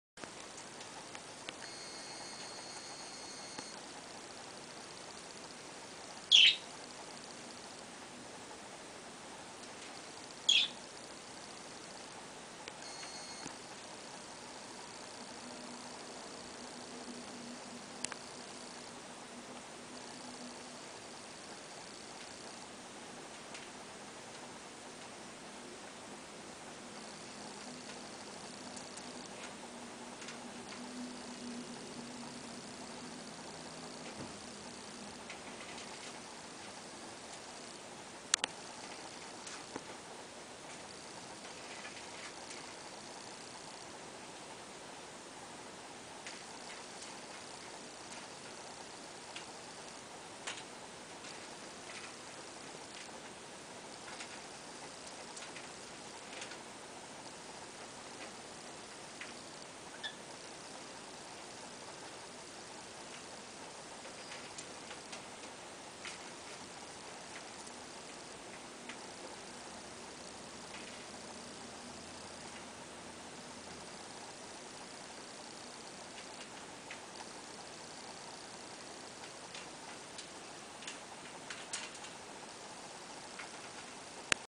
My budgies & cockatiel playing infront of the mirror!
OMG. i couldn't help filming these adorable little birdieeees they are so cute... (L) playing infront of the mirror and all xD, they must have loved themselves or thought that it was another bird infront of them xP